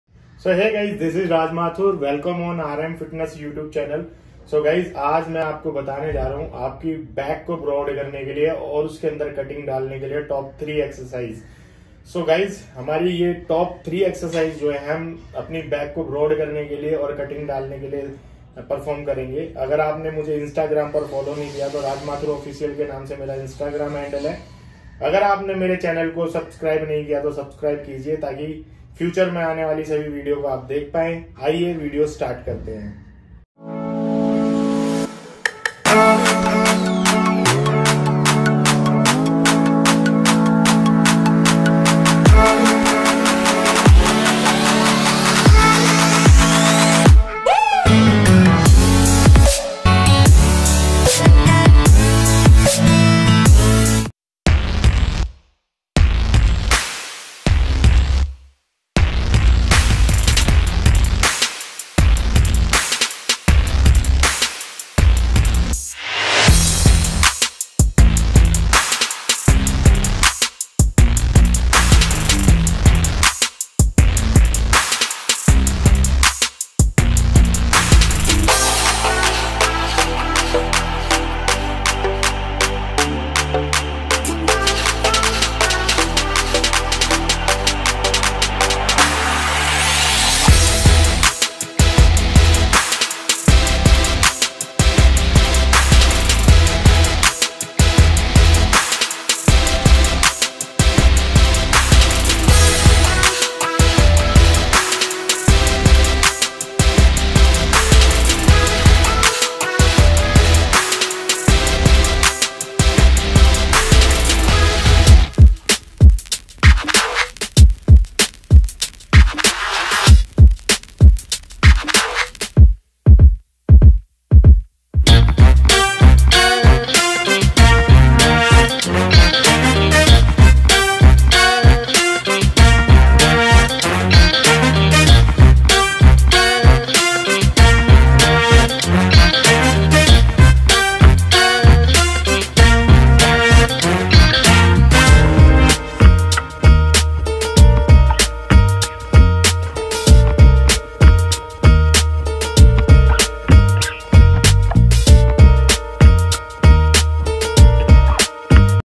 Back Workout by|raj Mathur Mr Asia | Bigback Back Exercise
That’s to 3 workout of back you can see, the back is quite complicated. Yet, there are two main types of movements that work the back: pulldown movements and rowing movements. Then there are power moves, like the deadlift, snatch and power clean. These moves are more of a whole body exercise; they work much more than the back.